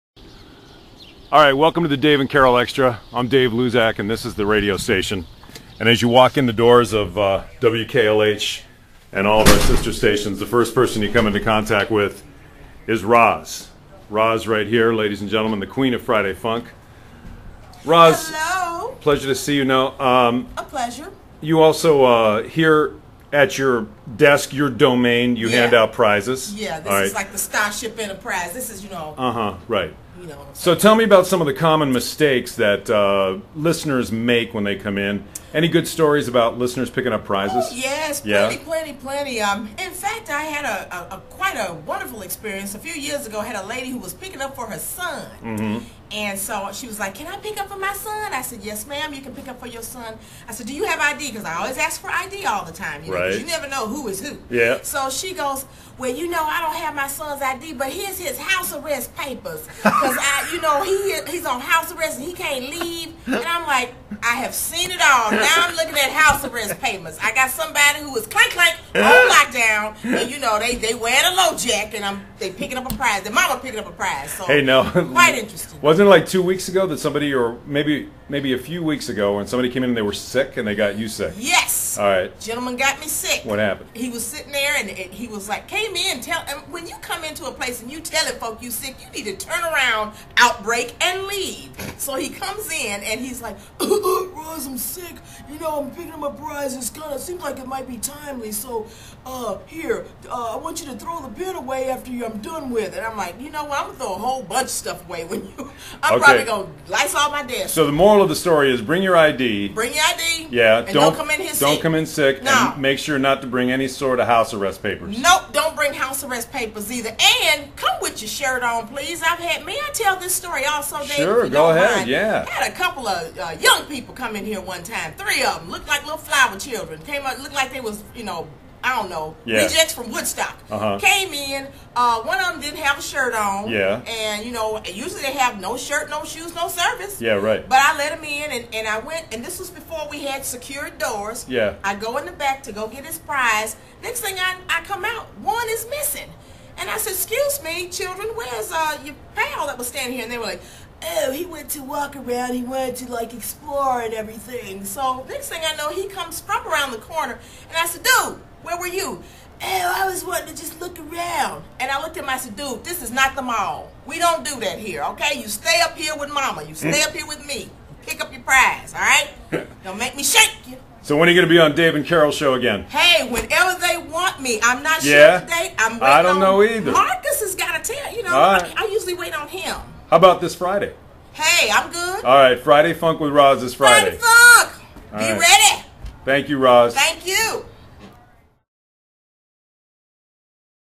D&C Extra: Tellin' Stories with Roz
Roz teases her upcoming Friday Funk apperance... and tells a few stories 'bout workin' at the front desk.